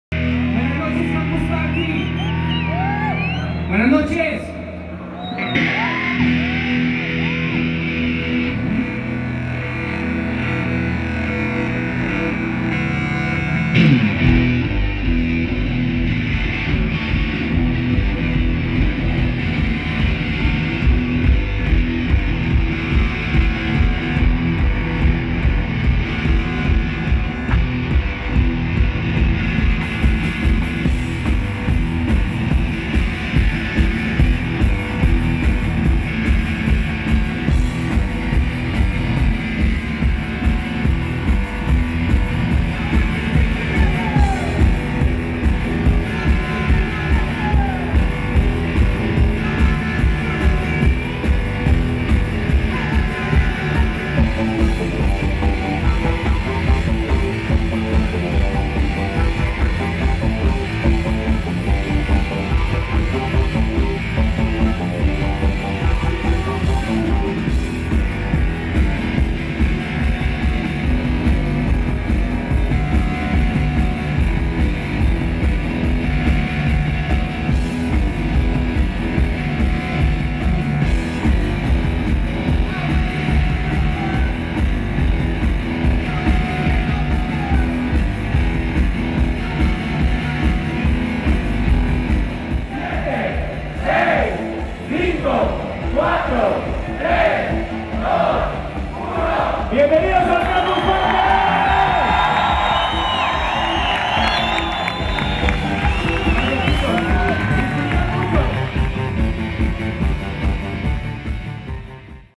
CPCO4 - Vídeo inauguración
Video de la inauguración que muestra en un breve resumen los acontecimientos sucedidos en este primer día.